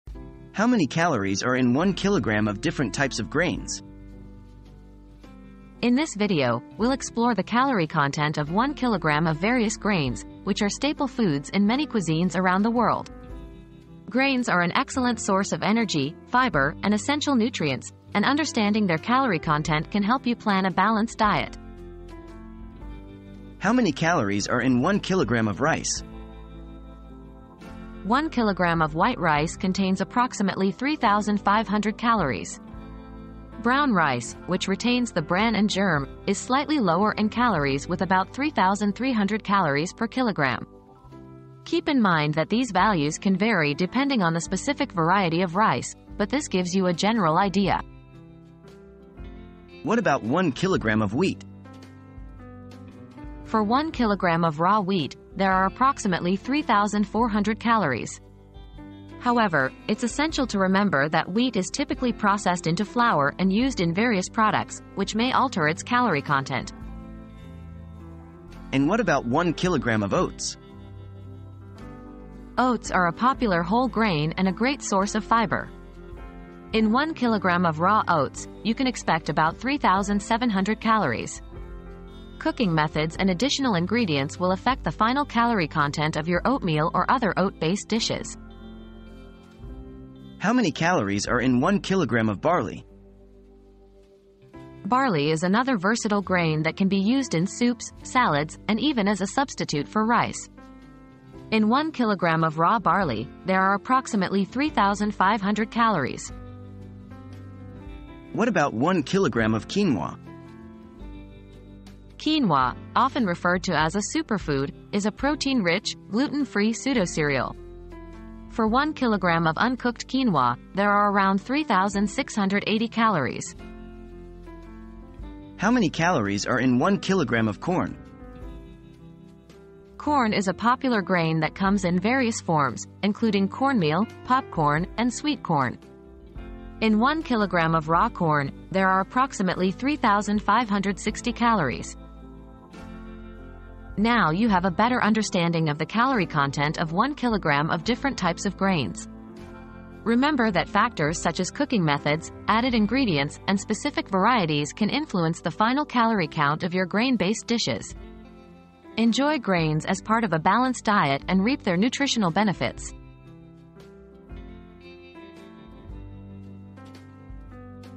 How many calories are in 1 kg of different types of grains?
How Many Calories are in 1 kg of Different Types of Grains? • Get ready to learn about the calorie content of 1 kg of different types of grains in this informative video! From rice to quinoa, find out how many calories are in your favorite grains and discover new ones to add to your diet. Understanding the calorie content of grains is key to maintaining a balanced diet, so don't miss out on this essential information!

00:00 • How many calories are in 1 kg of different types of grains?
00:23 • How many calories are in 1 kg of rice?
00:49 • What about 1 kg of wheat?
01:08 • And what about 1 kg of oats?
01:30 • How many calories are in 1 kg of barley?
01:48 • What about 1 kg of quinoa?
02:05 • How many calories are in 1 kg of corn?